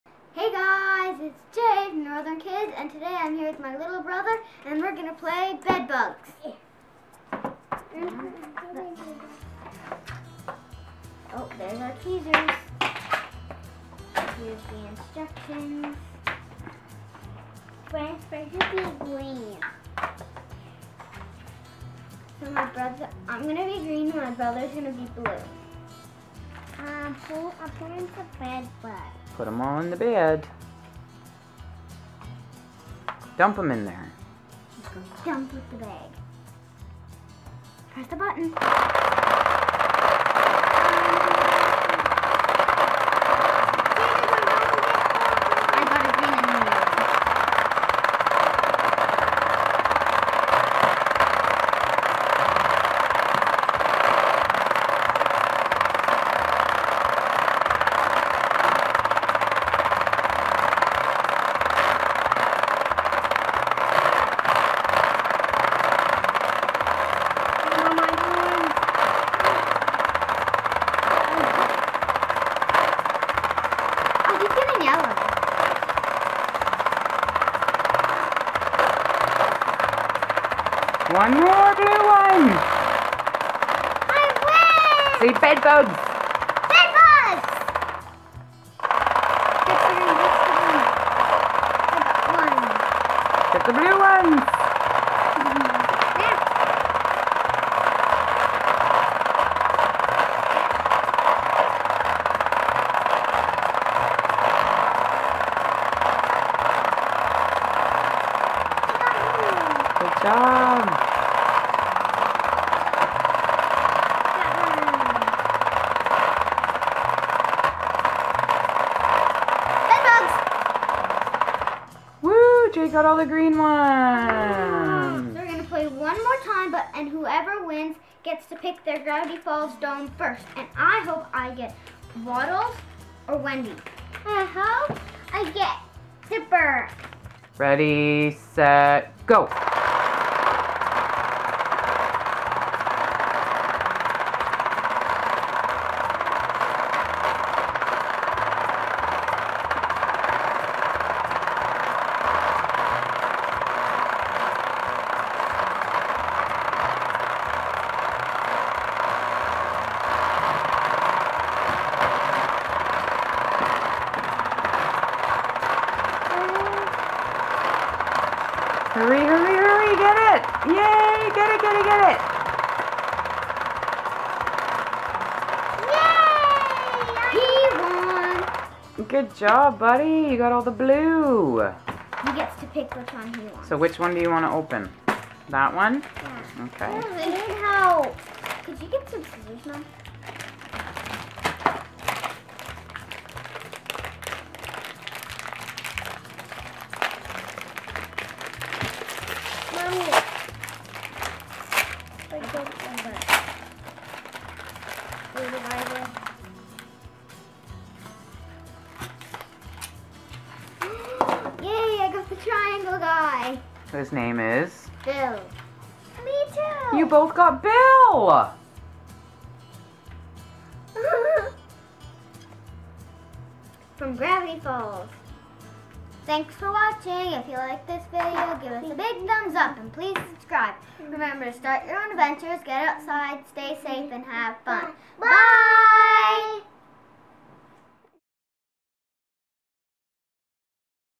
Kids Play Bed Bugs Game Gravity Falls Prize Opening
Watch us have fun playing Bed Bugs by Hasbro Games and open our Gravity Falls Prize.
Viewers may want to turn the volume down. The bouncing bugs are noisy.